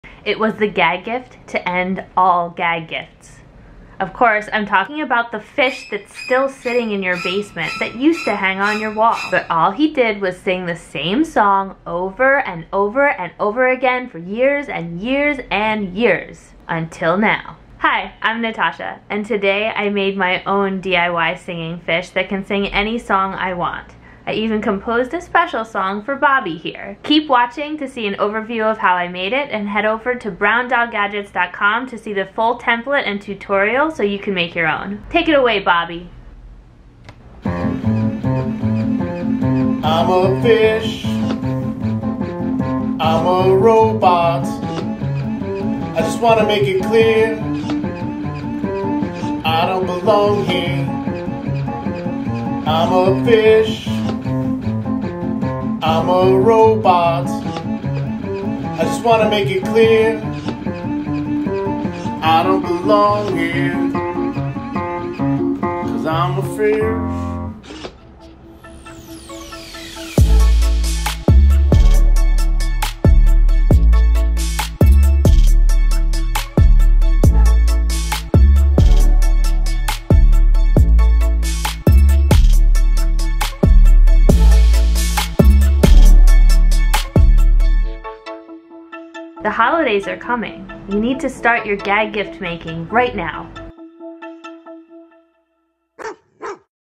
DIY Animatronic Singing Bass Fish with micro:bit and Brown Dog Gadgets bit:board
Get the full template and tutorial to make this dancing robot fish